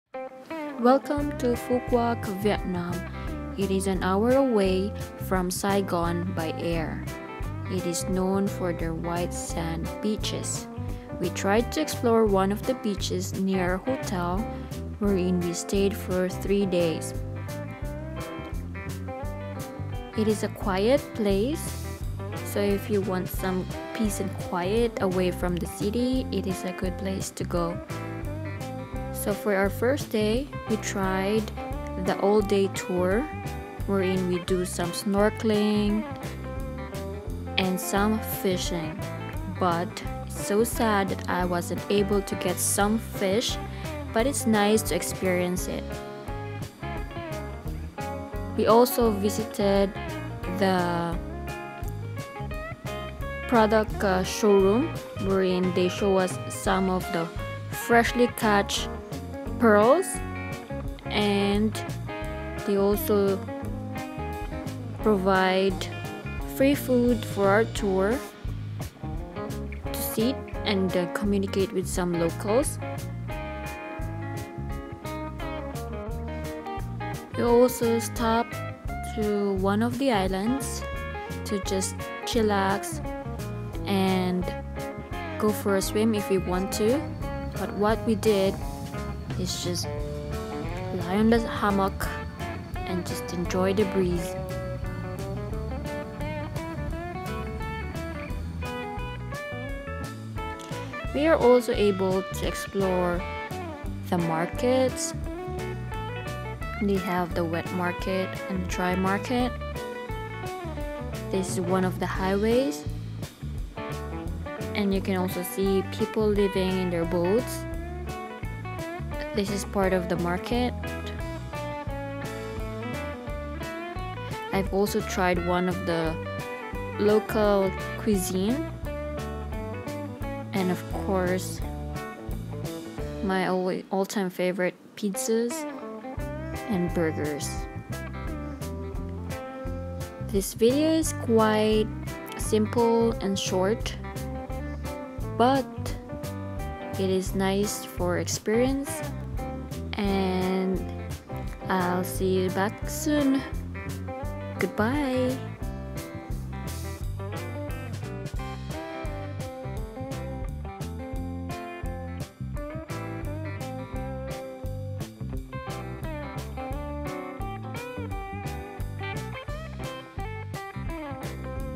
Things To Do in Phu Quoc Vietnam
Phu Quoc, Vietnam 🇻🇳 is an hour away from Saigon by air.

They are known for their white sand beaches where we stayed for 3 days.

On our first day, we do an all day tour wherein we do snorkeling, fishing, swimming.

The tour includes free food. We stopped to one of the islands to relax.

They also have wet and dry market and we are able to see some people living in their boats.

If you want peace and quiet away from the city, this is a good place to go and to explore.